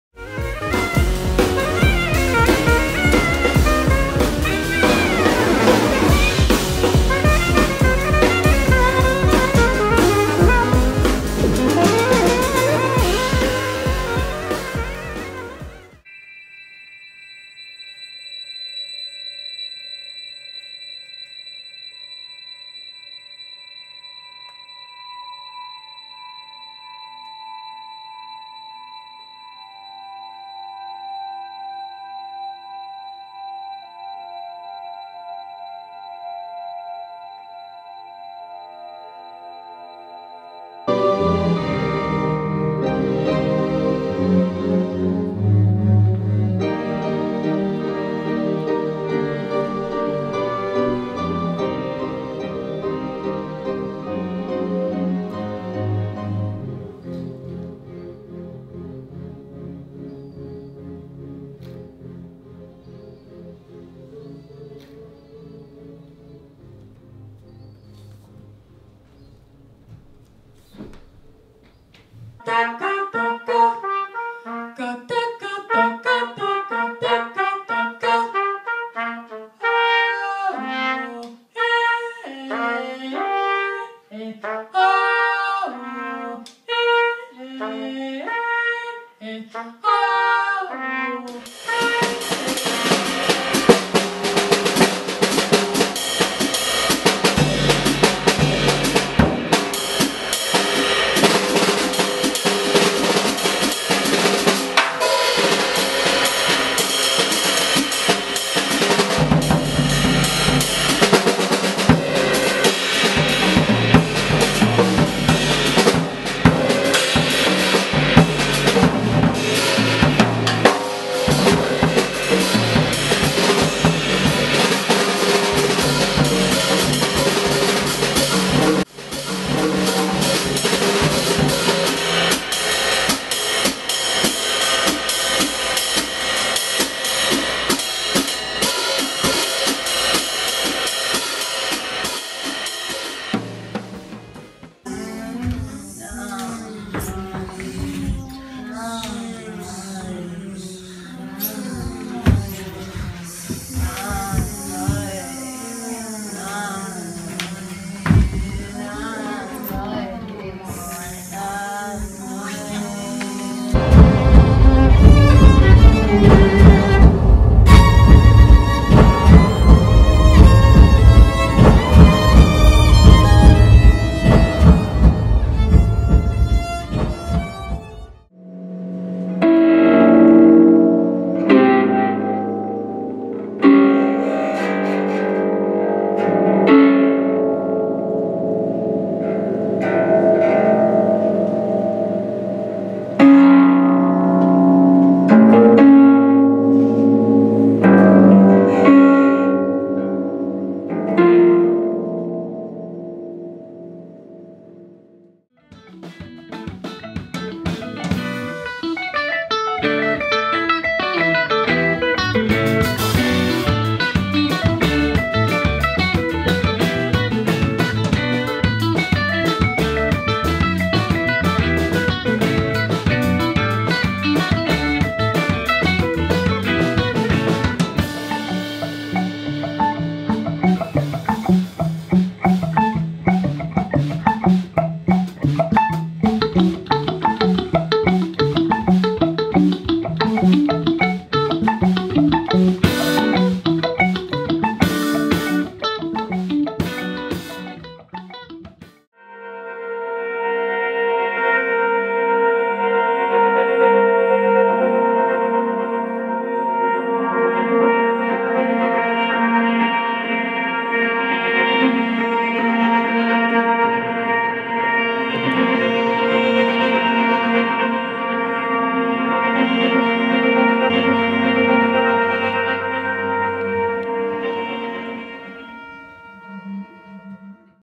Home Audio 2011-2016 Sample
This video contains clips from past shows between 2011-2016 from Home Audio's monthly performance series in Brooklyn, curated by Mara Mayer.